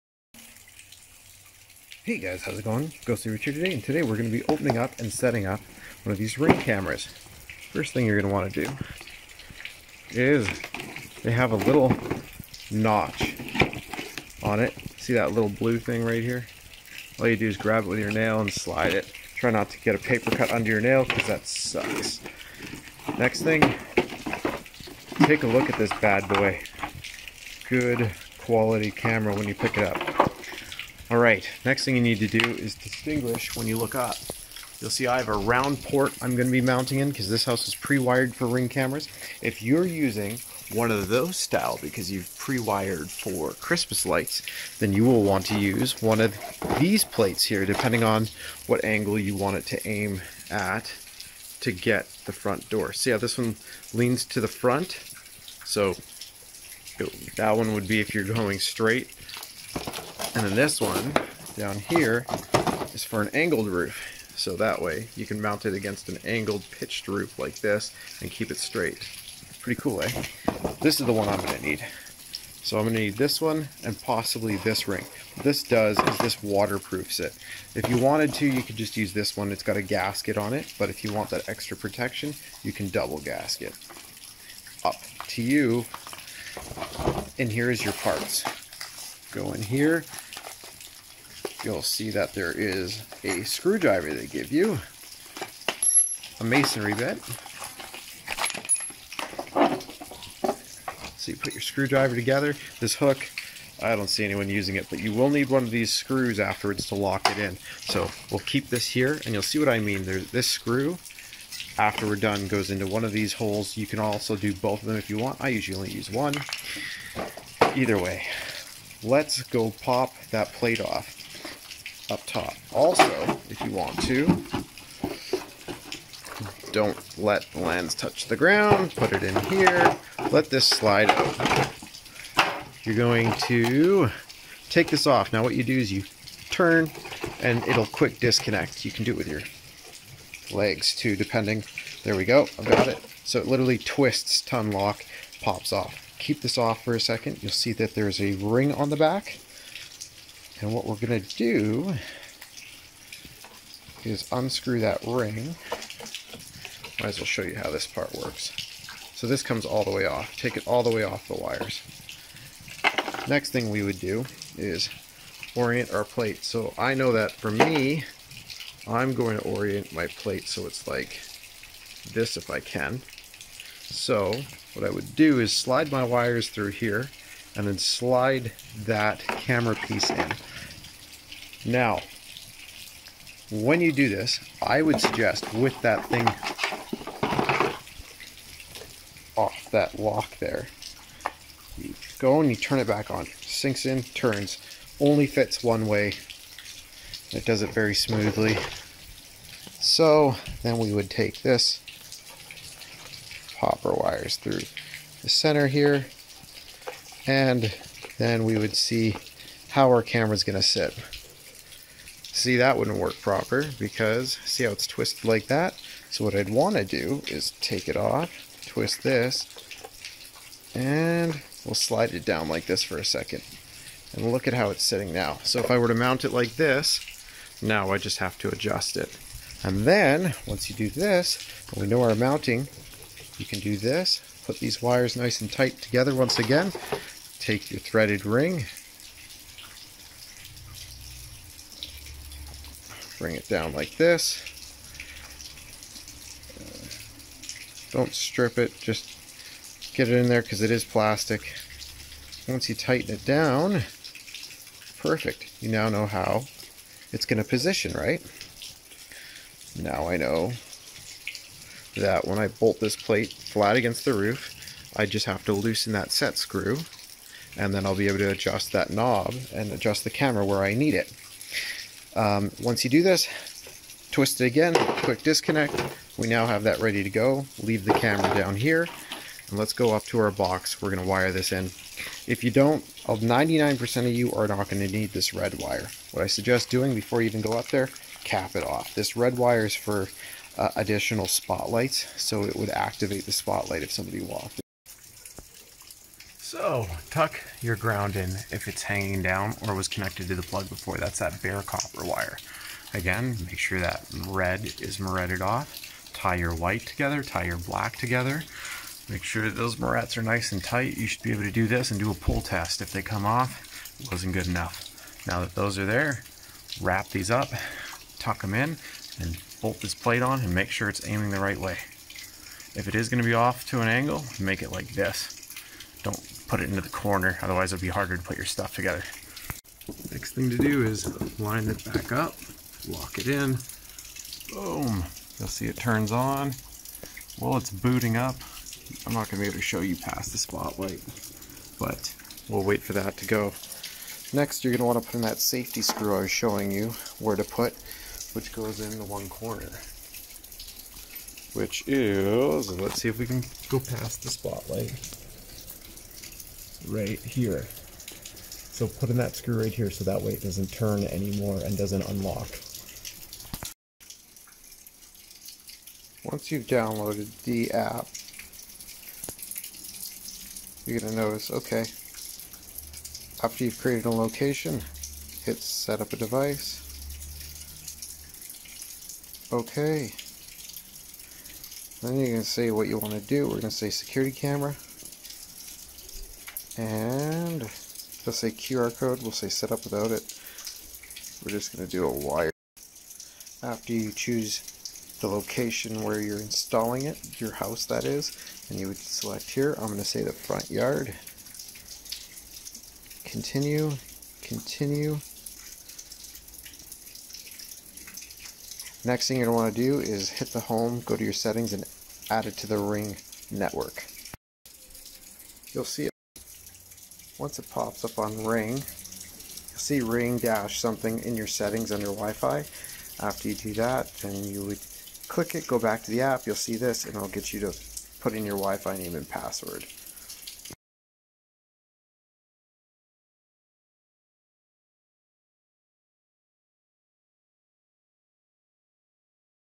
HOW TO INSTALL RING WIRED SPOT LIGHT CAMERA
Just a quick  how to on wiring and mounting a ring camera and explaining the different wires, i hope this helps you wire and install your camera and set it up through the app.